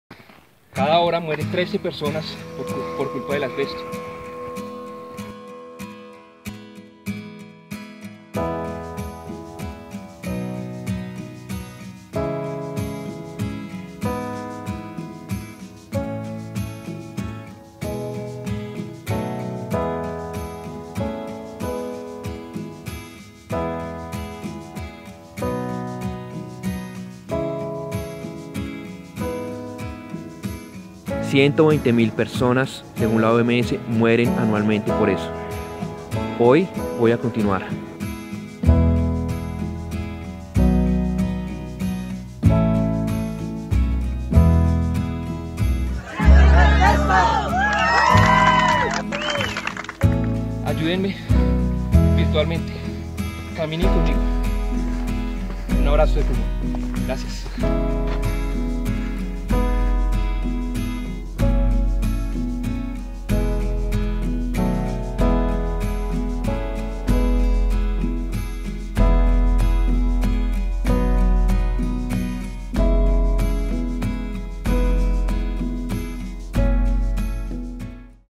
Un metro por cada víctima del asbesto
El asbesto causa la muerte a cientos de miles de personas cada año y Ana, esposa de Daniel, fue una de ellas. A causa de esto Daniel decidió caminar 60km, pero al llegar al objetivo lo duplicó y recorrió 120km. Así continúa la lucha de su esposa para lograr una ColombiaSinAsbesto.

Lo apoyan más de 27 MIL colombianos que ya reclamaron a Eternit Colombia que abandone esa fibra cancerígena.